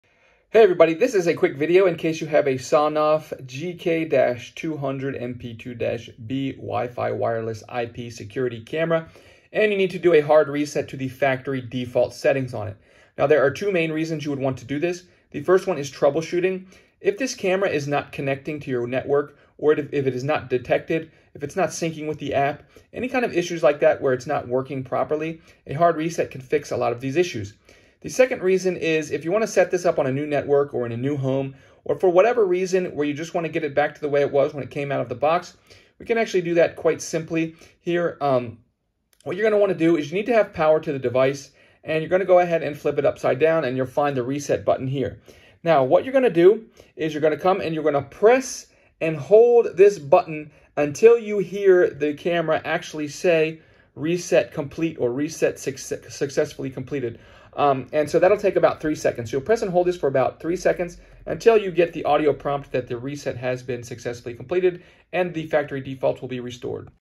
Hard Reset Sonoff WiFi Security Camera
How to troubleshoot or setup starting with a hard reset a Sonoff SONOFF GK-200MP2-B WiFi Security Surveillance Camera to factory default settings if it is not working or not connecting or if it is frozen or not responding, the factory reset is a good troubleshooting step. You can also restore the factory settings to setup your device as new. Found in the SONOFF GK-200MP2-B Camera user manual.